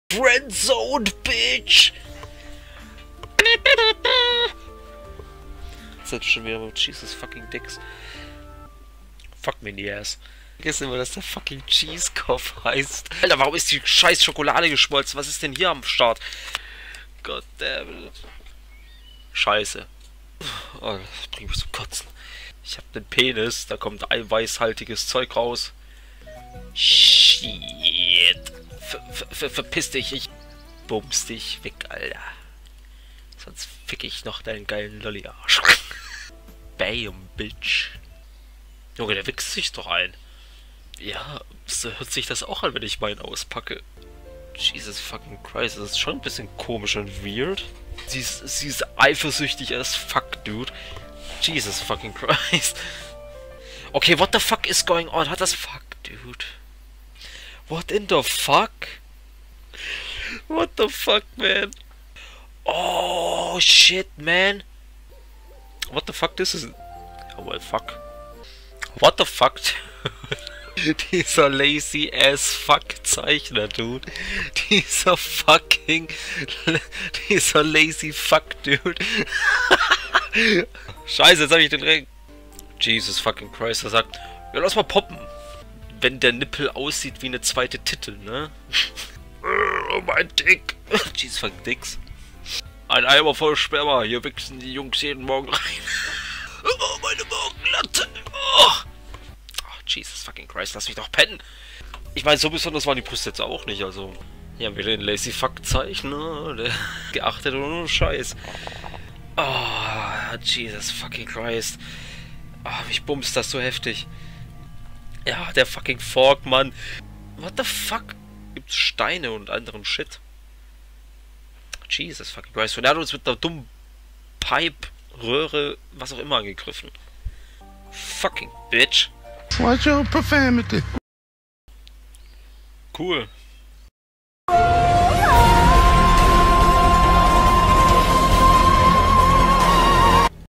Cman Flucht über 2 Minuten lang
Björn (25) ist stolz auf seine seefahrenden Vorfahren, um dass zu zeigen redet er wie ein Matrose.

"Jesus Fucking diks" ~Cman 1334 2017